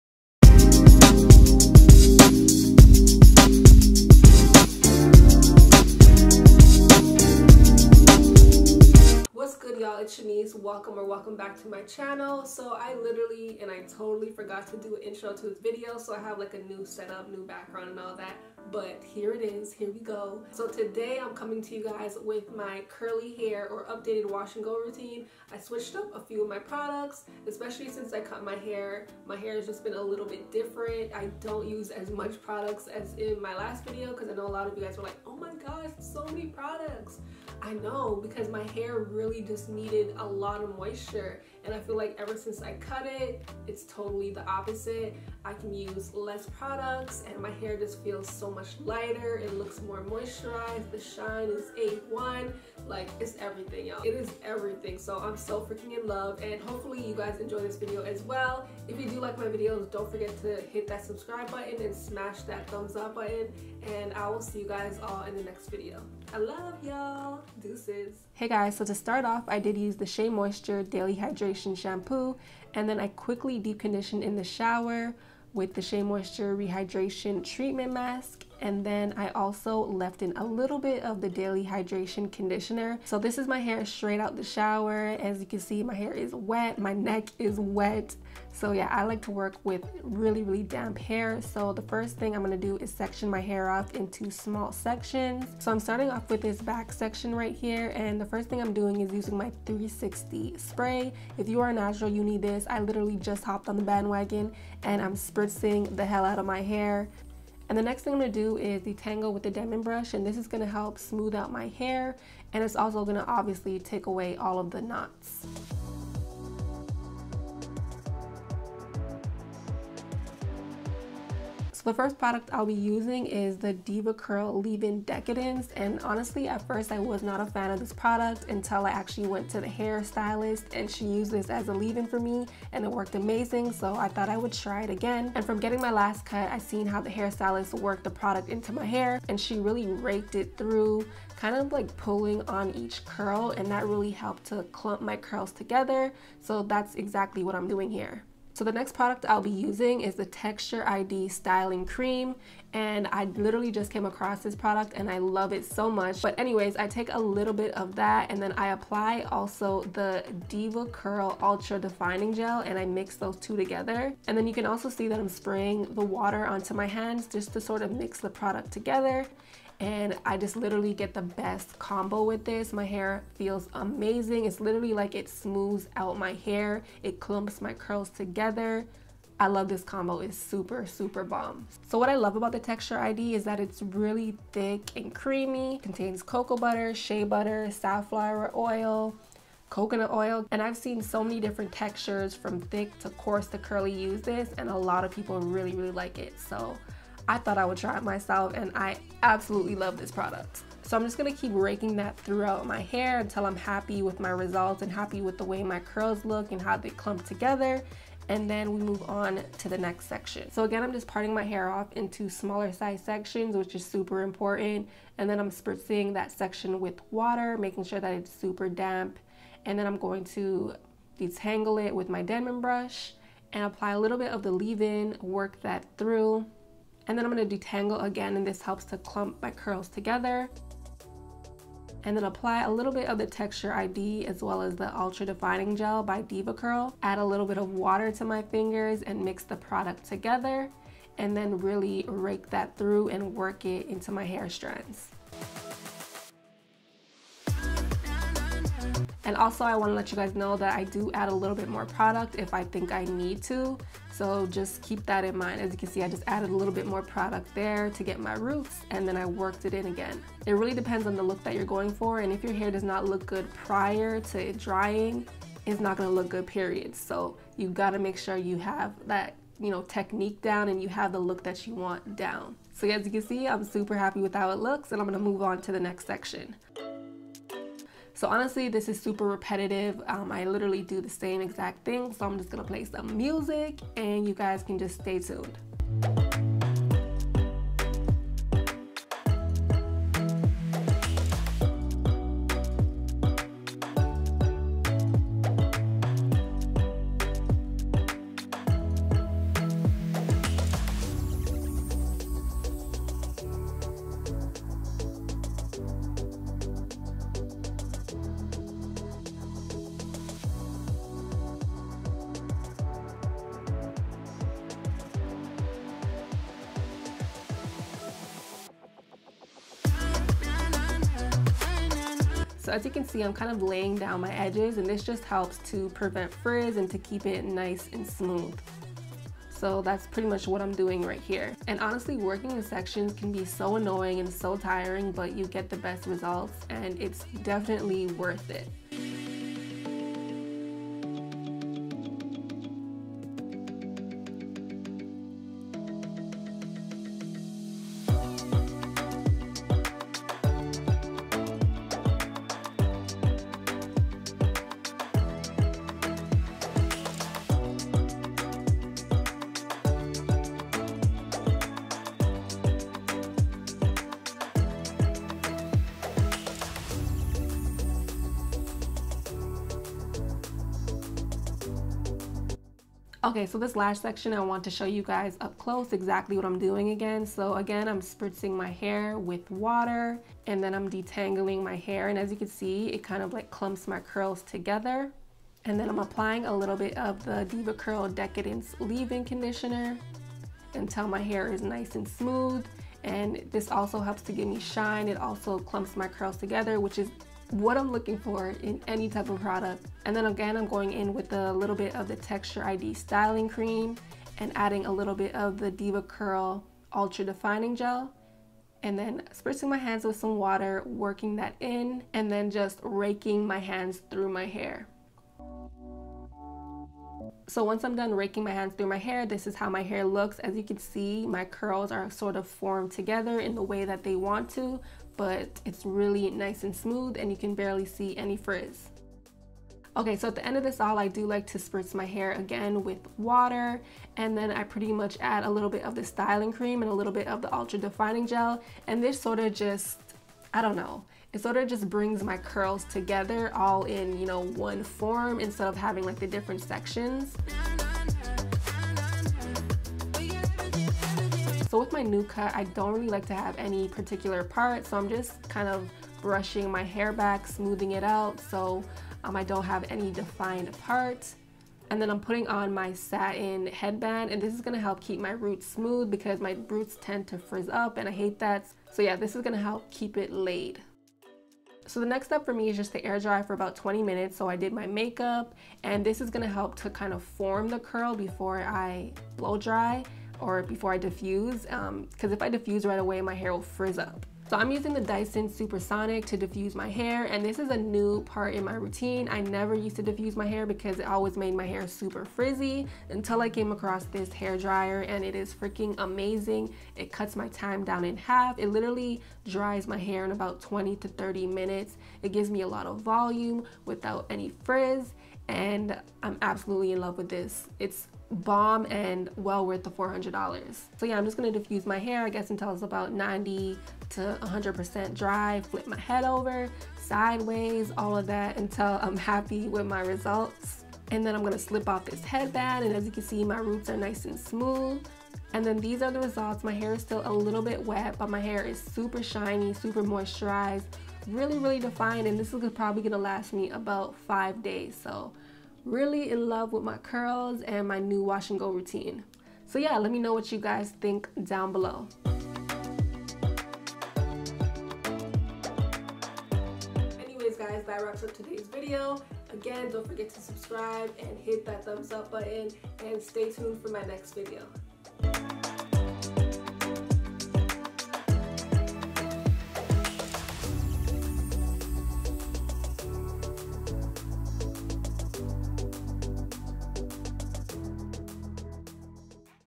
MY CURLY HAIR ROUTINE ON MY NEW HAIRCUT | NO FRIZZ!!
Hey everyone!! So ever since I cut my hair my Wash N Go or Curly Hair Routine has changed tremendously!! I finally got routine down with little to no frizz and i'm absolutely in love!!

Products Used:
Shea Moisture Shampoo
Shea Moisture Deep Conditioner
Shea Moisture Conditioner
Deva Curl Leave in Decadence
Texture ID Styling Creme
Deva Curl Ultra Defining Gel
Dyson Supersonic Diffuser

🎥 CAMERA + EDITING ↓
C a m e r a : Canon 80d
L e n s : Sigma 30mm f/1.4 ART lens